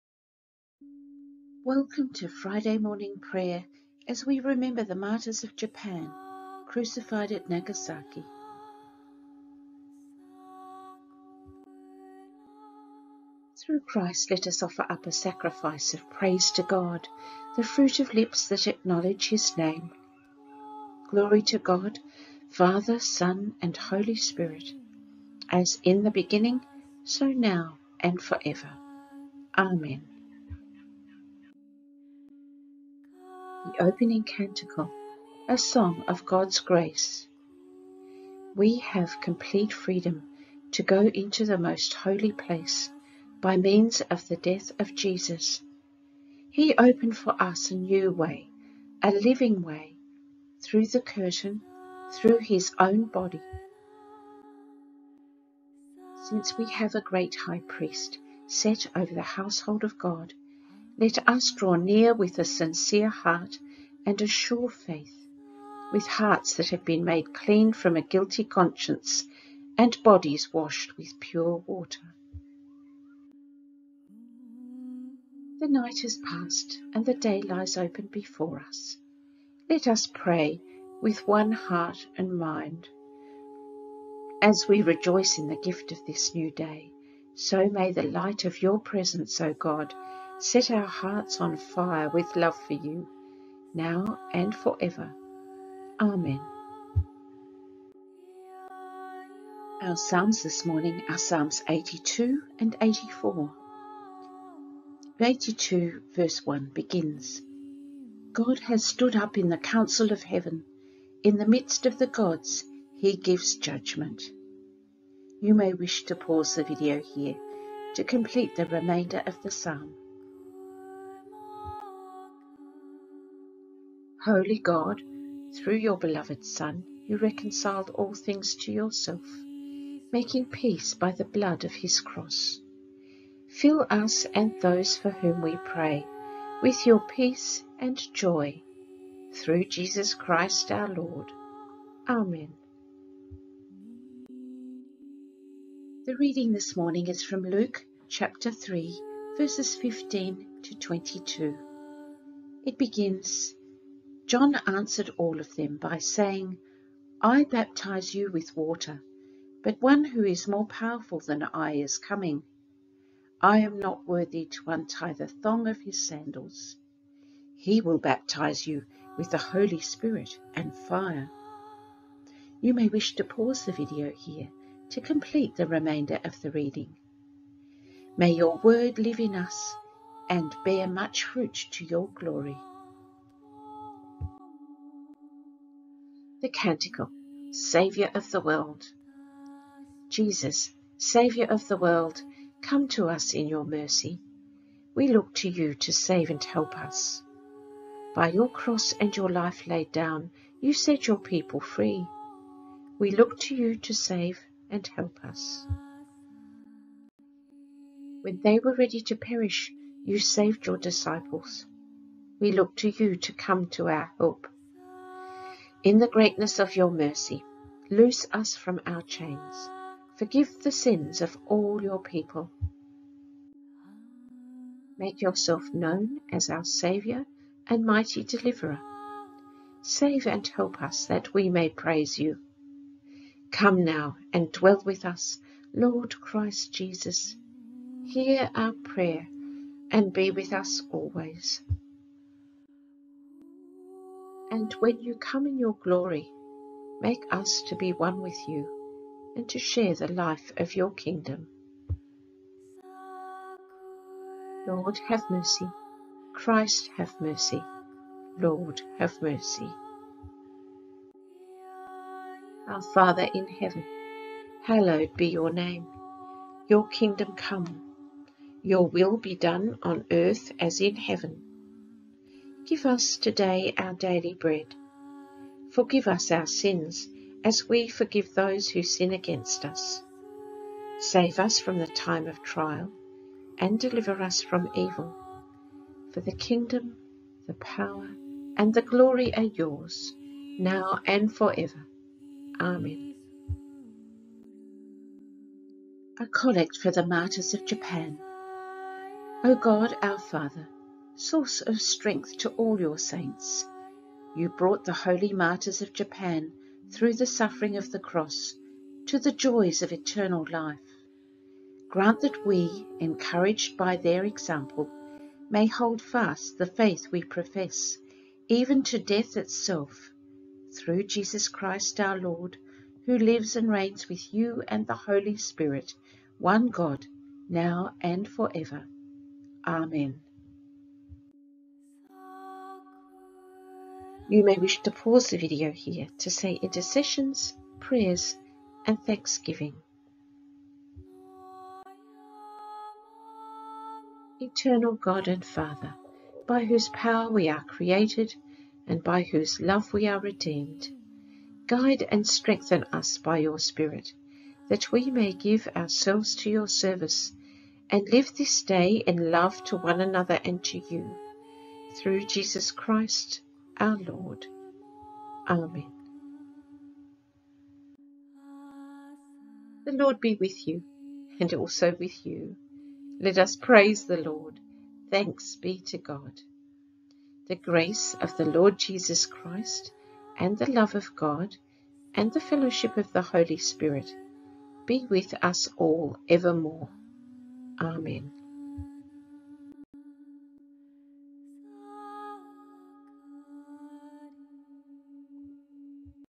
Friday Morning Prayer 21 02 05
Welcome to Friday Morning Prayer as we commemorate the Martyrs of Japan 1597.
The first missionary, the Jesuit Francis Xavier arrived in Japan with two companions in 1549 and was followed by Portuguese missionaries who drew tens of thousands more Japanese to the faith. By the end of that century there were about 300,000 baptized believers in Japan.
The first victims were six Franciscan friars and twenty of their converts who were crucified at Nagasaki, February 5, 1597.
The priests were expelled in the 17th century by military leaders who feared their growing influence.
By 1630, what was left of Christianity in Japan was driven underground.
For the next 250 years hiding their religion became a matter of life and death for Japanese converts, as Christianity was banned and Japan closed itself off from the outside world.
Some suspects were forced to trample on holy images to prove they were not Christians. Many went into hiding or disguised their faith.
Those who refused to give up Christianity faced being burned to death, crucified, or slowly drowned.
It is remarkable that 250 years later in 1865, when Japan lifted its self-imposed isolation, a group of nervous peasants approached a Christian missionary working in Japan. One woman whispered “ours hearts are the same as yours,” revealing the previously unknown existence of what turned out to be tens of thousands of the original Christians’ descendants. Many descendants, men and women known as Kakure Kirishitan (hidden Christians in Japanese), had preserved through the generations without priests, a vestige of Christian faith.
As we remember the martyrs of Nagasaki, may their dust and ashes lead to resurrection.
In peace and with every blessing of goodness,
Rev’d Mandy

Readings:
Psalms 82; 84
Luke 3.15-22